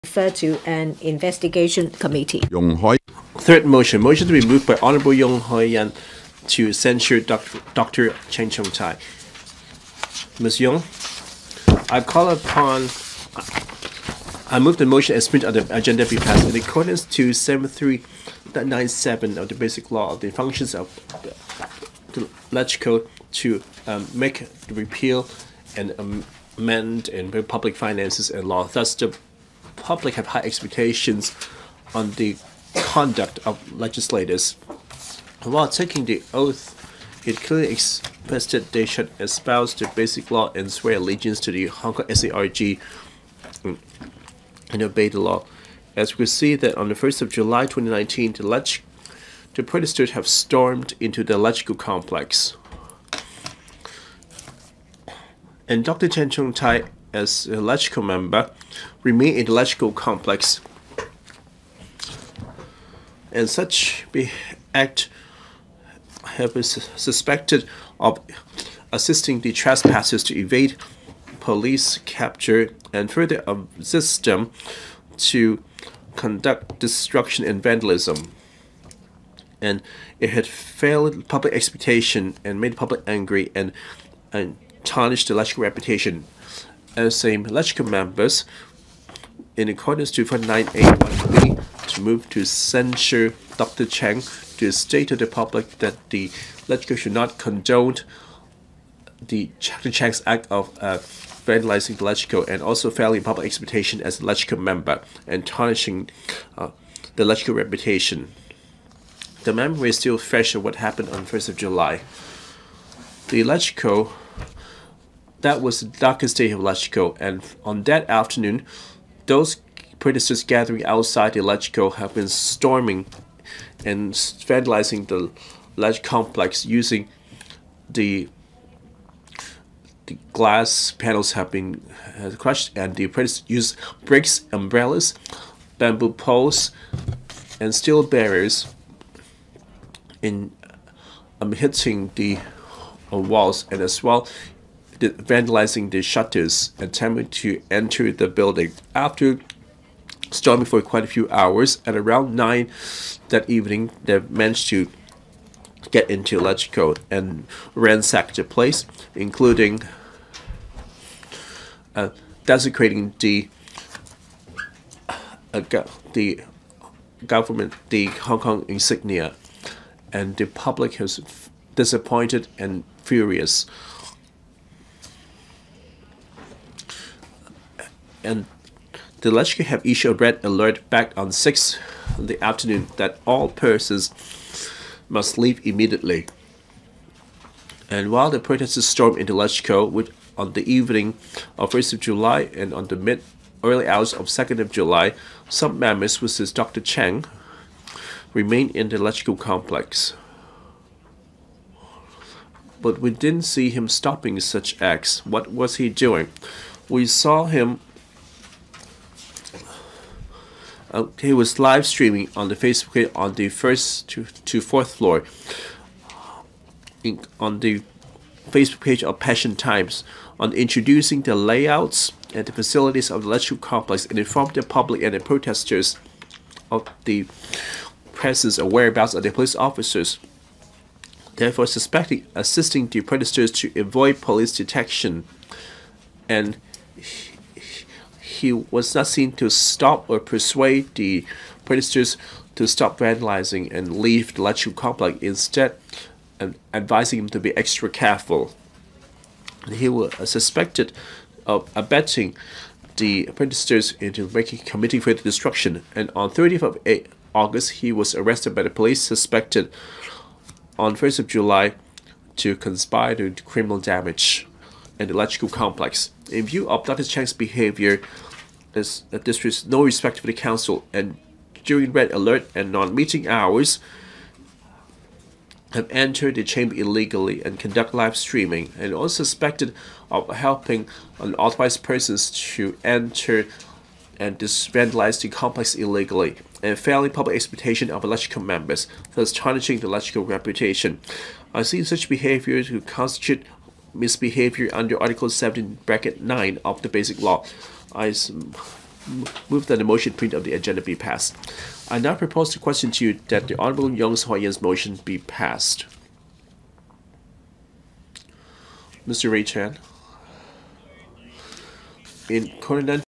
Council meeting (2019/12/11)-V.- Mot./u R49B(1A) of the RoP to censure Dr Hon CHENG Chung-tai
V. Members’ Motions

Motion under Rule 49B(1A) of the Rules of Procedure to censure Dr Hon CHENG Chung-tai

Hon YUNG Hoi-yan to move the motion in Appendix 8

(This motion jointly signed by Hon Mrs Regina IP, Hon WONG Ting-kwong and Hon POON Siu-ping)